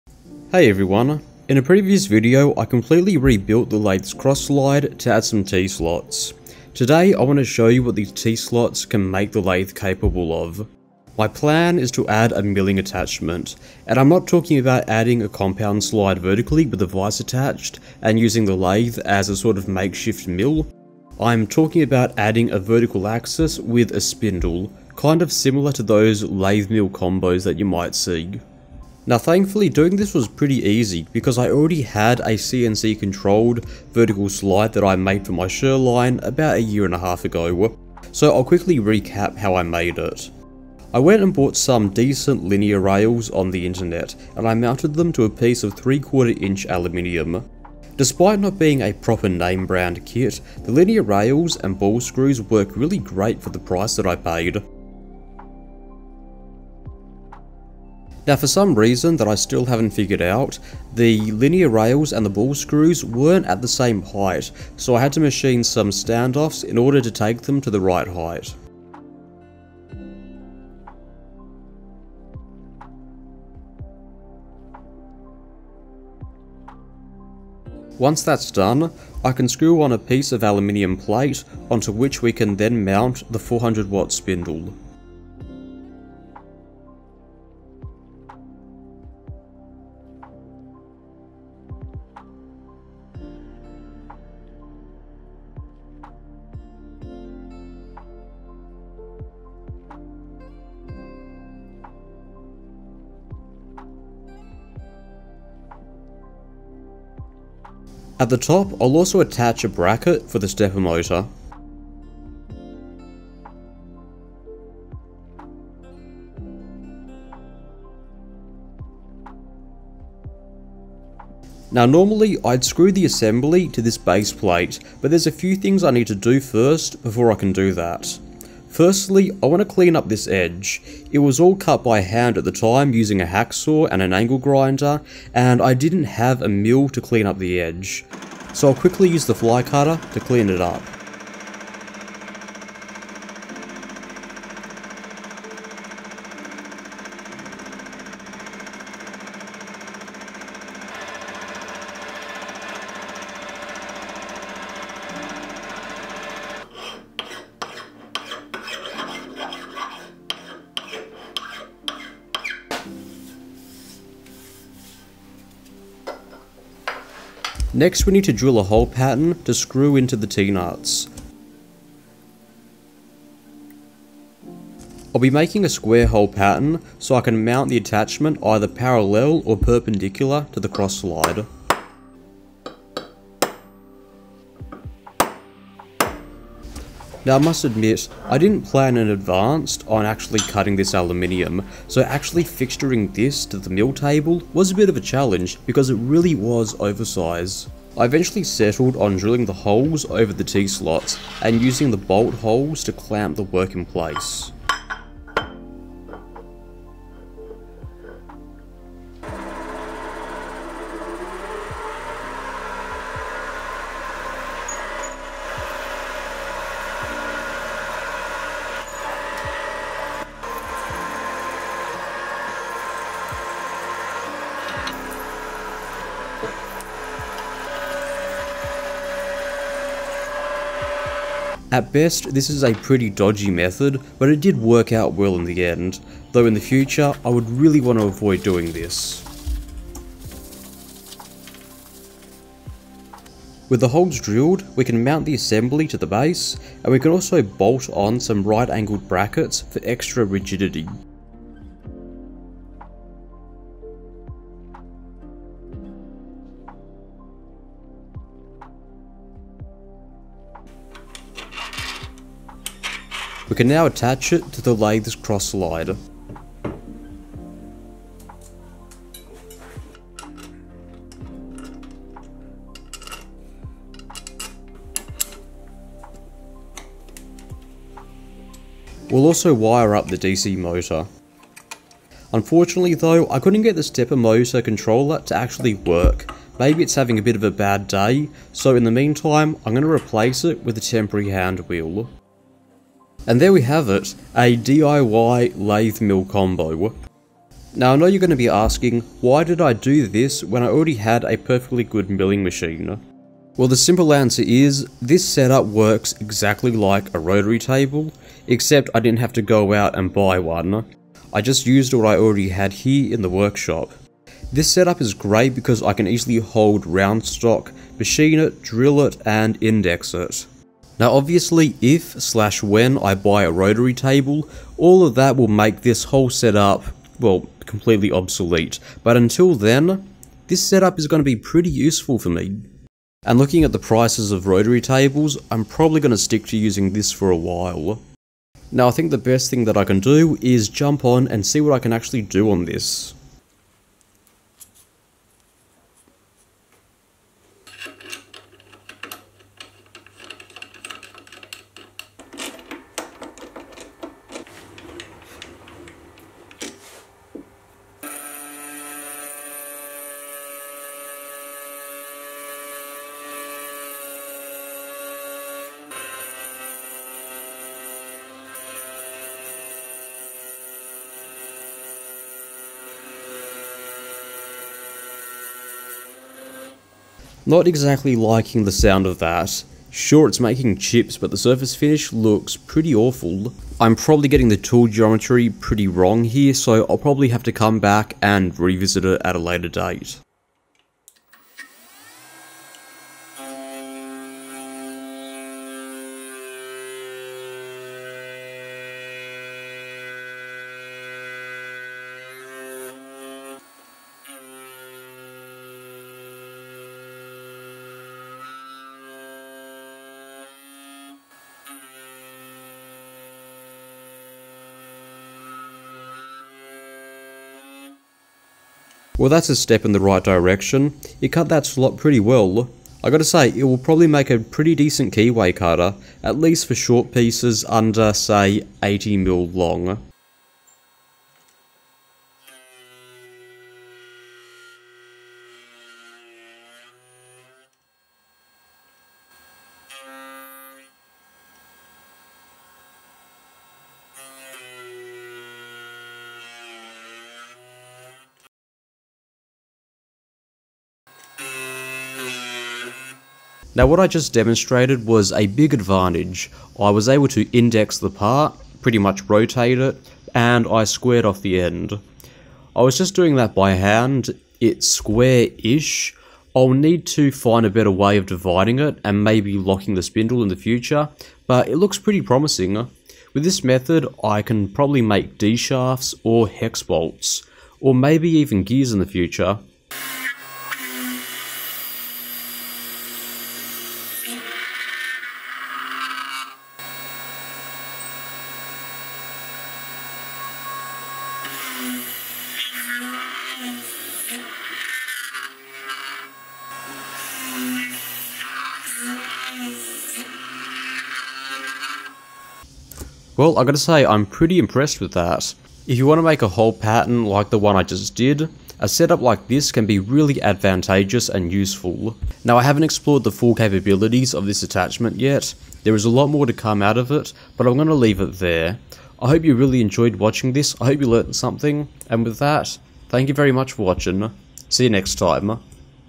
Making A Lathe Milling Attachment | Lathe Mill Combo Mod
G'day everyone,

In this video I will be making use of the t-slots that I made in a previous video where i rebuilt the mini lathe's cross slide. A modification that I have wanted to try for a long time is to make a milling attachment for the lathe. Having a milling axis will allow me to cut kay ways, index using the lathe head and eventually allow me to cut my own spur gears.

In this video I will be making and modifying my existing CNC controlled vertical axis to work with the mini lathe, and doing some basic milling operations.

Lathe: Sieg c3 7x14 mini lathe
Mill: Sieg x2.7l Mini Mill

Timestamps
0:00 - Intro
0:40 - Making The Milling Attachment
4:52 - Testing The Milling Attachment